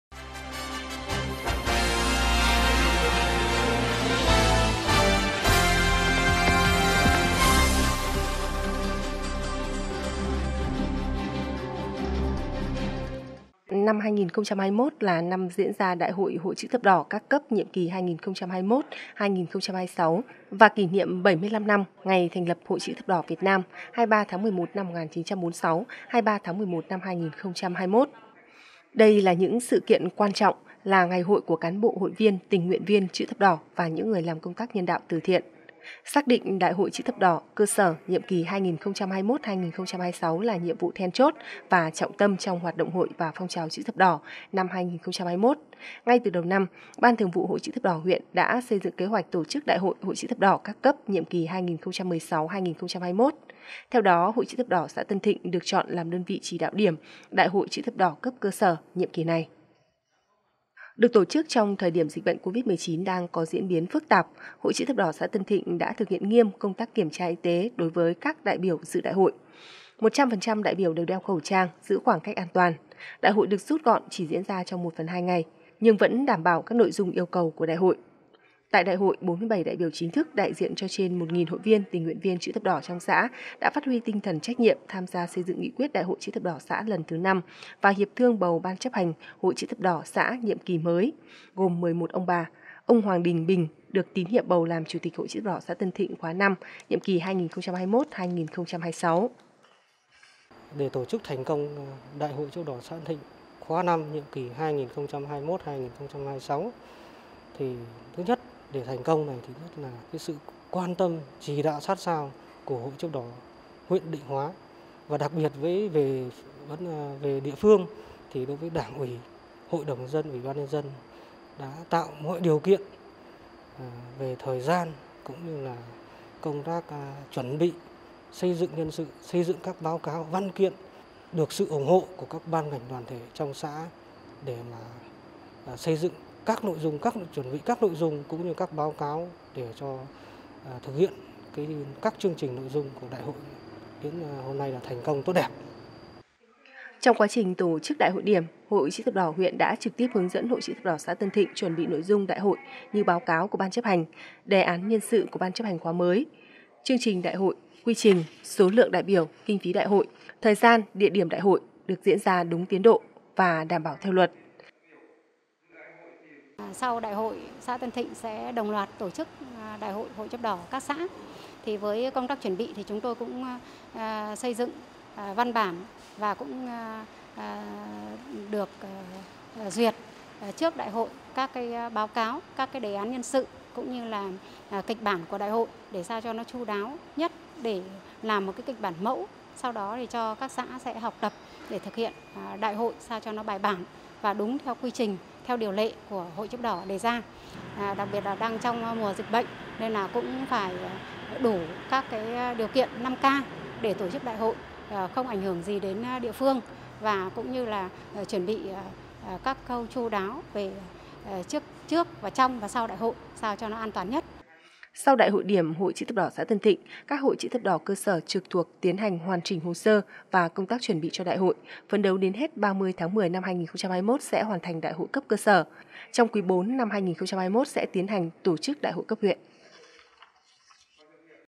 Ghi nhân từ thành công của Đại hội đại hội đại biểu Hội chữ thập đỏ xã Tân Thịnh khóa
Ghi nhân từ thành công của Đại hội đại hội đại biểu Hội chữ thập đỏ xã Tân Thịnh khóa V, nhiệm kỳ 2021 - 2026